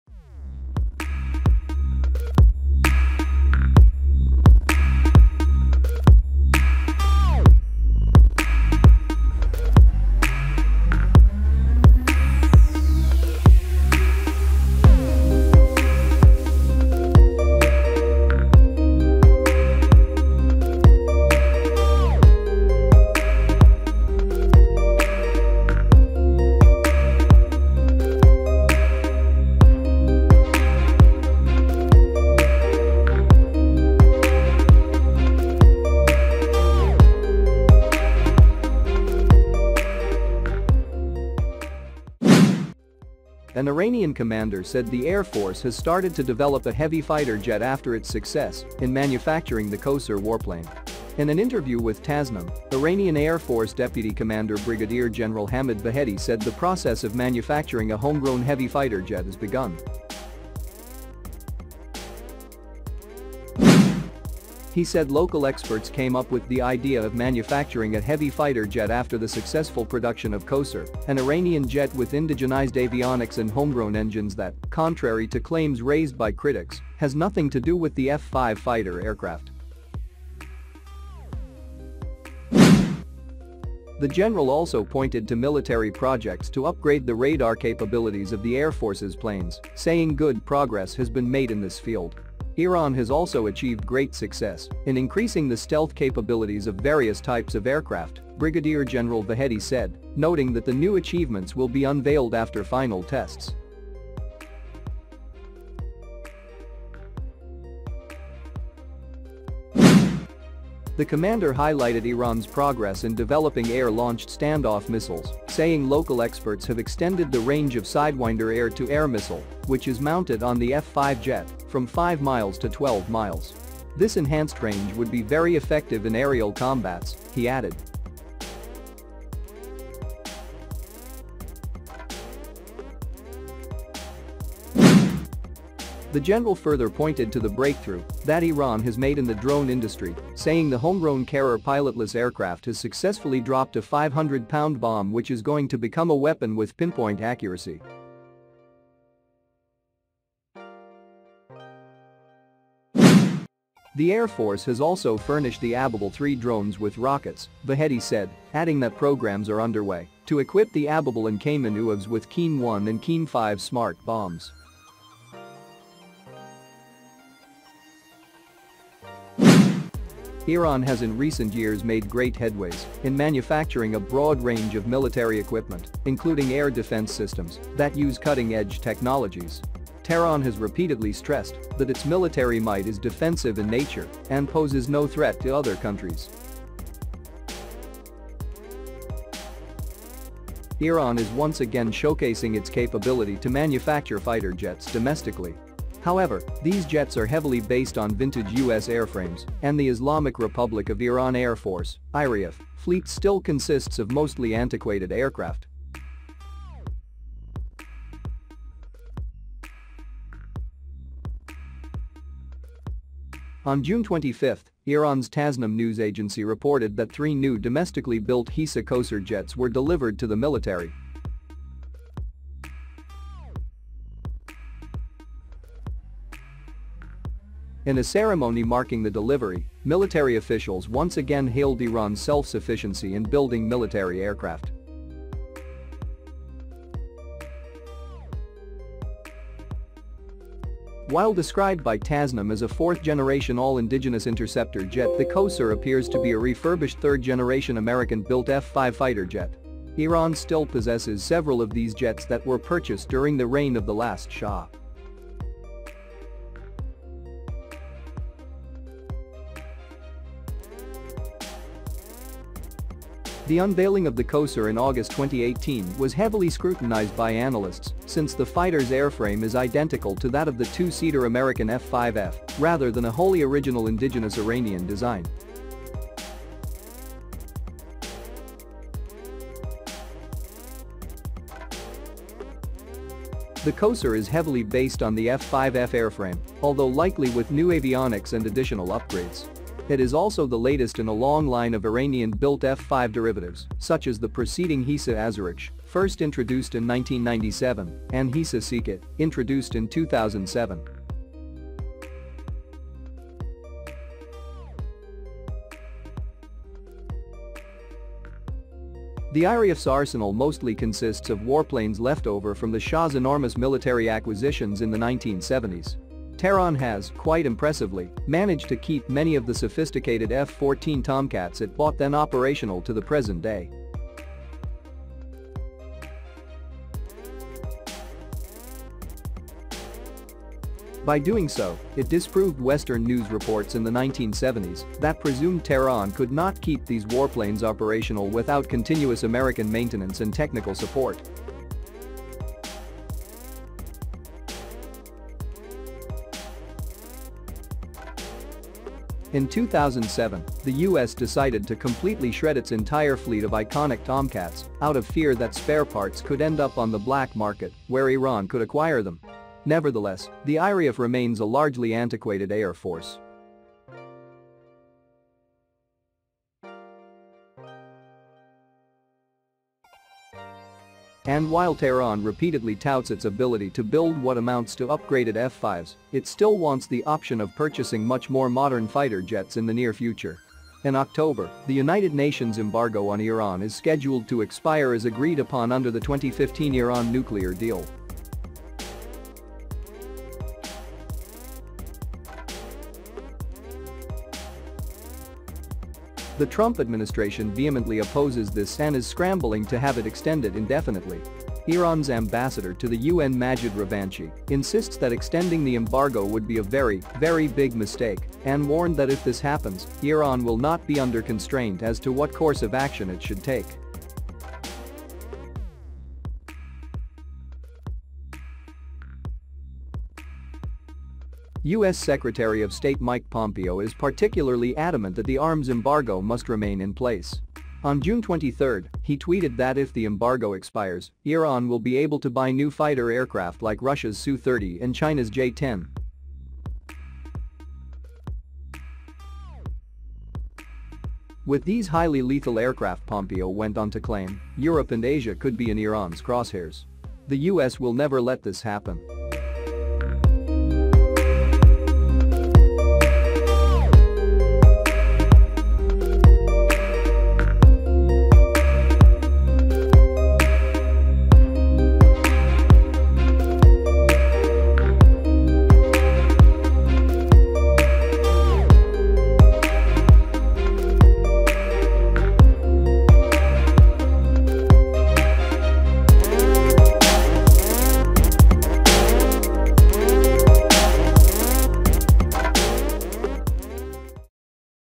Iran Started To Develop A Heavy Fighter Jet with Indigenized Avionics And Homegrown Engines
An Iranian commander said the Air Force has started to develop a heavy fighter jet after its success in manufacturing the Kowsar warplane.

In an interview with Tasnim, Iranian Air Force Deputy Commander Brigadier General Hamid Vahedi said the process of manufacturing a homegrown heavy fighter jet has begun.